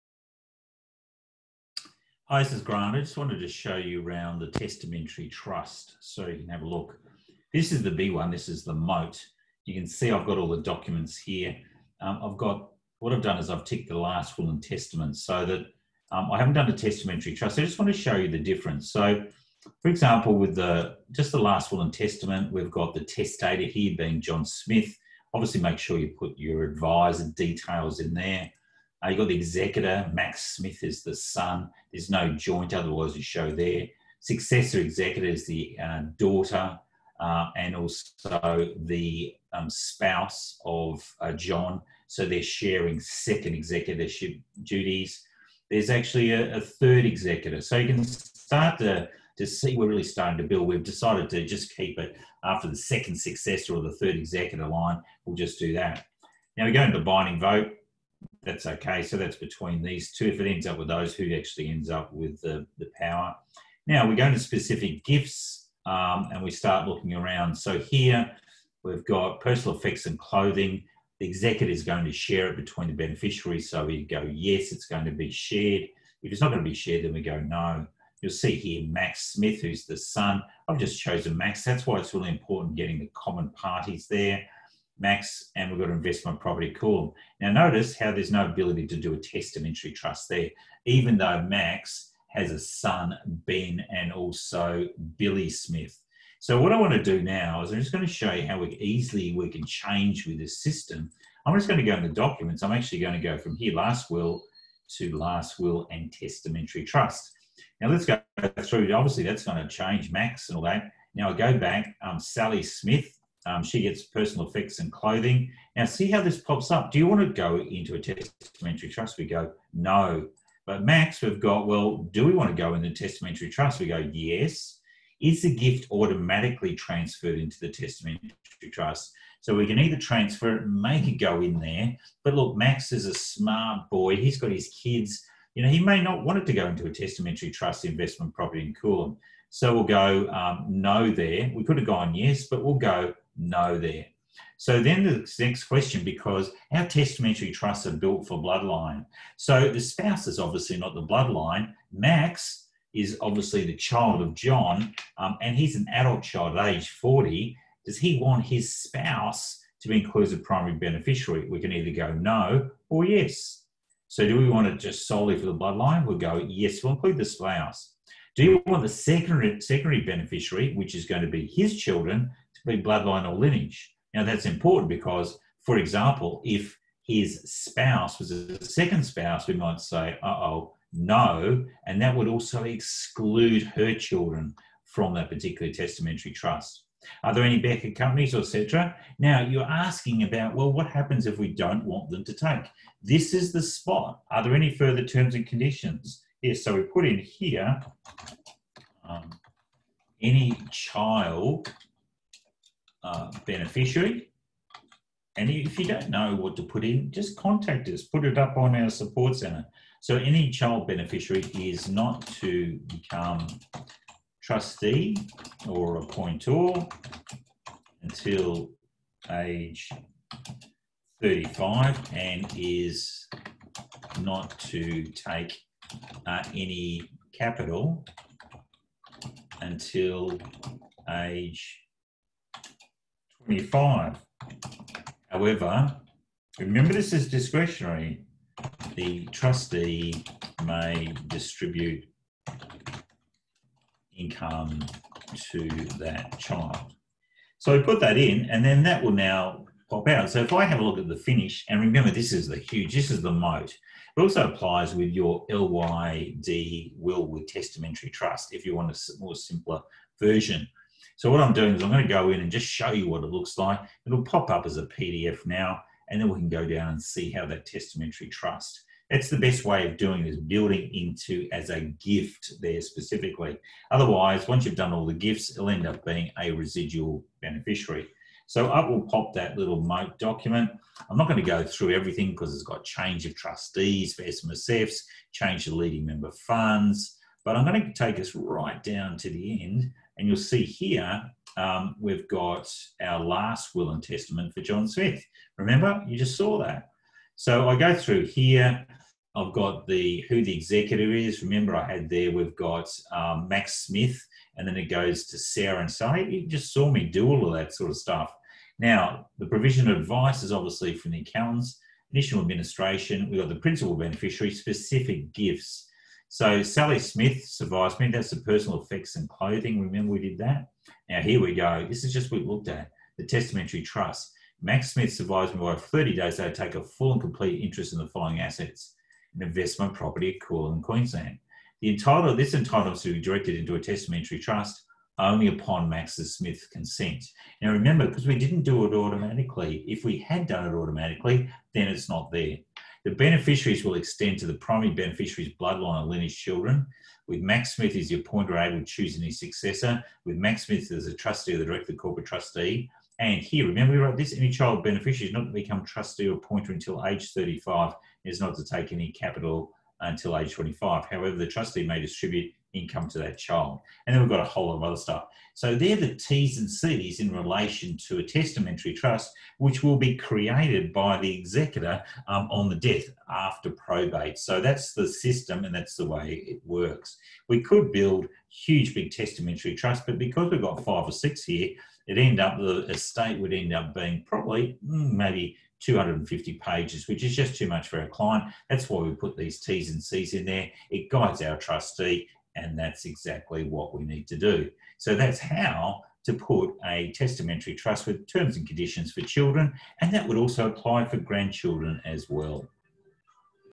Placing limits on Child Beneficiaries in a Testamentary Trust
In this video Grant Abbott from LightYear Docs shows users how to impost limits on trusteeship, being an appointor or accessing capital for child beneficiaries in a testamentary trust.